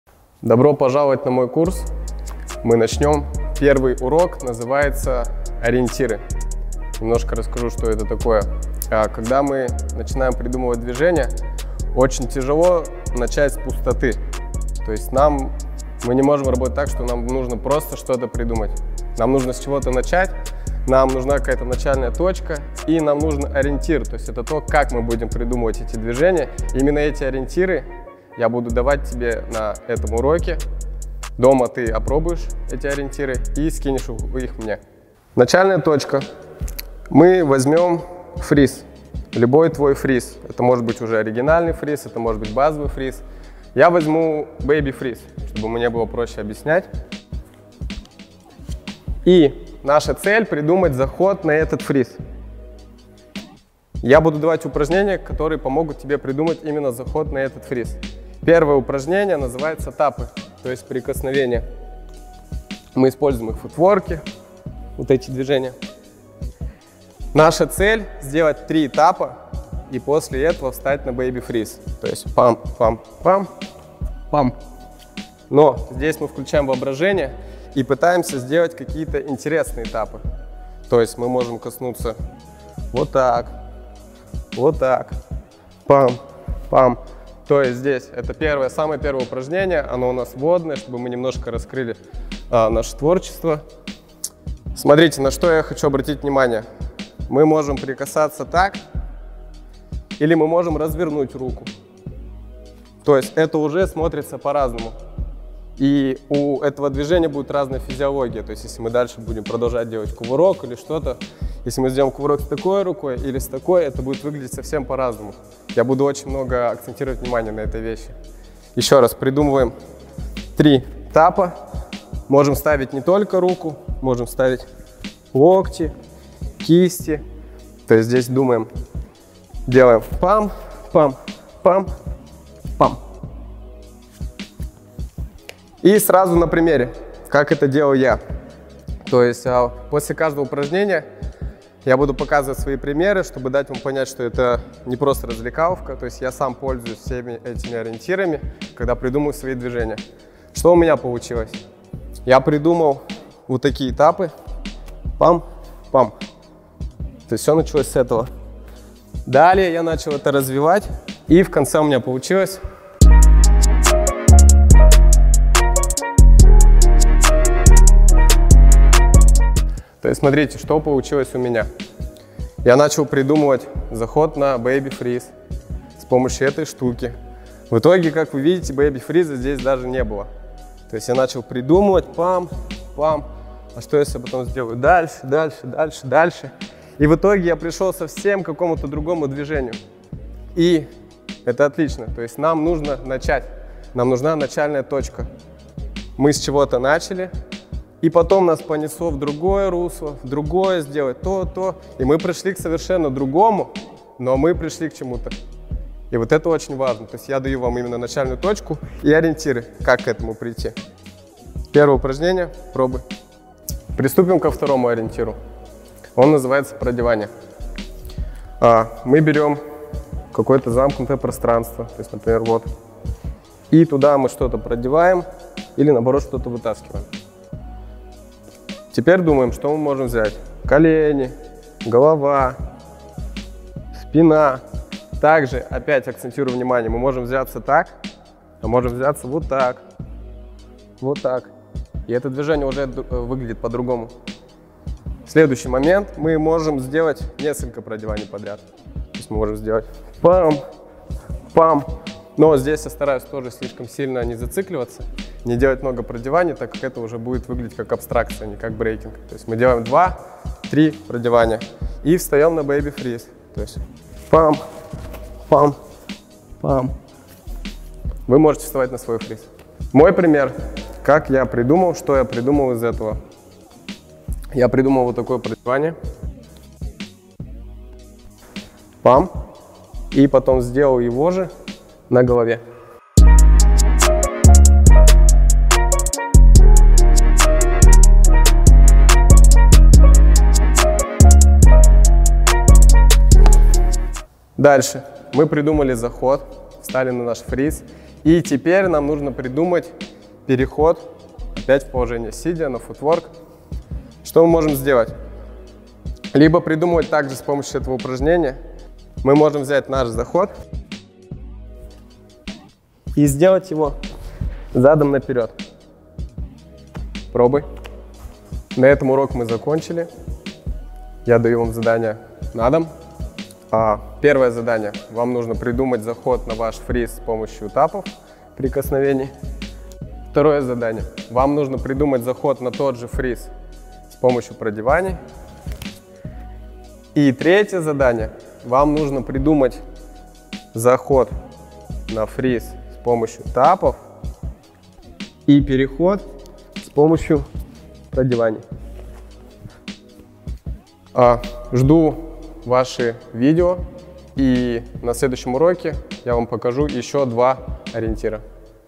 EP1 : Motives: Taps & Threads / Course 'CREATIVITY' by AMIR (Predatorz) | BBOY.ONLINE EXCLUSIVE
First episode of exclusive bboy.online course by Amir (Predatorz/PDVL) "CREATITY". First episode is called "Motives". To create something new you need a starting point - a motive. Learn two motives - taps and threads. How to take these motives and build a new creative stuff from the scratch.